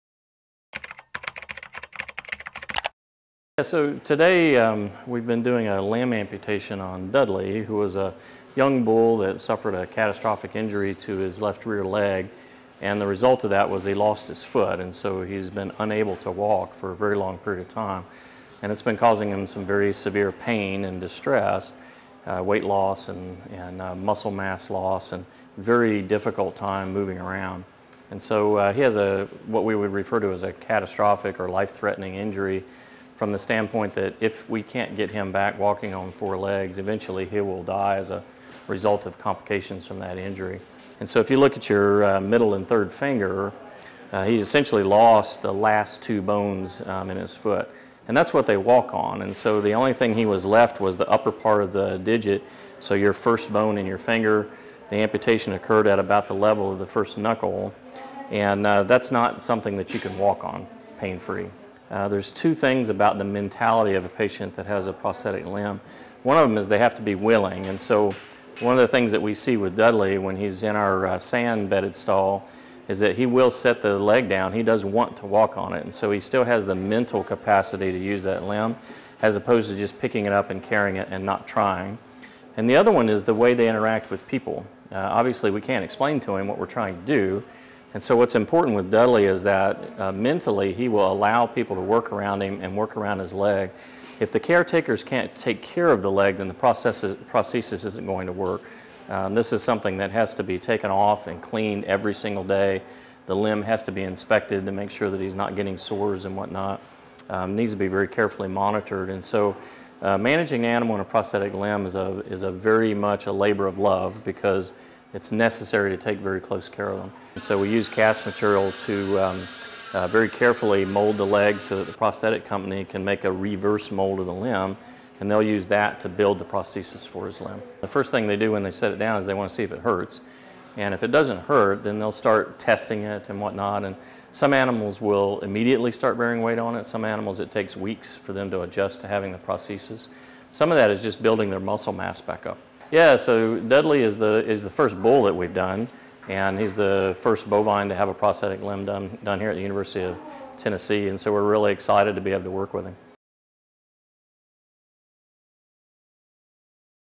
Rescued bull has foot amputation at UT College of Veterinary Medicine
University of Tennessee College of Veterinary Medicine Dr. David Anderson performs a foot amputation on 1-year-old bull Dudley, fitting him with a temporary prosthetic, until one can be made specifically for him. (Video by Amy Smotherman Burgess/News Sentinel)